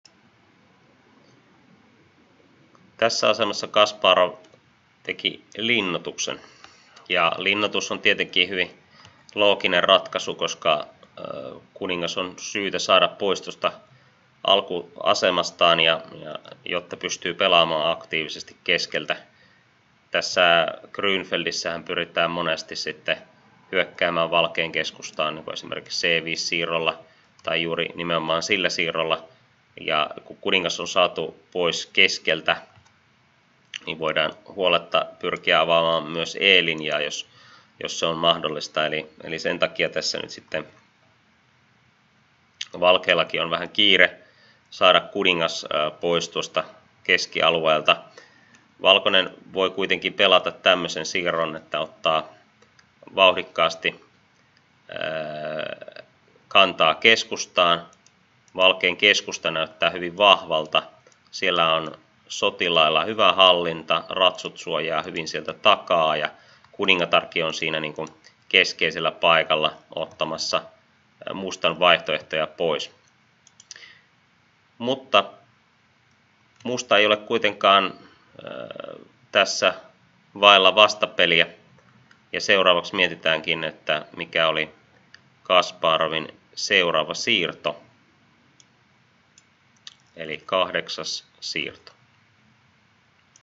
Maailmanmestarin siirtoja: Garry Kasparovin 7.siirto
Tämä videosarja käy läpi maailmanmestari Garry Kasparovin erästä hienoa kilpapeliä siirto kerrallaan. Kansainvälinen mestari Mika Karttunen selostaa pelin ideoita parhaansa mukaan. Johdannon jälkeen pelistä julkaistaan siirtopari päivässä, eli seuraajalla on mahdollisuus miettiä maailmanmestarin siirtoa seuraavaan videoon saakka.